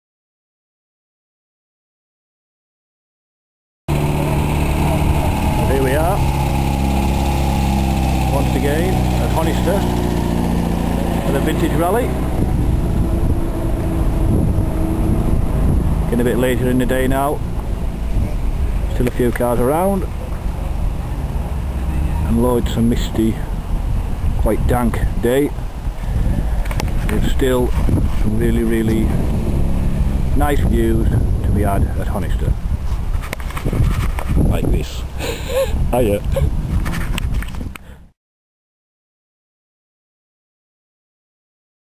Honister Slate Mine - Vintage Car Rally
Vintage Car Rally @ Honister Slate Mine with a cameo from Martin Nicholson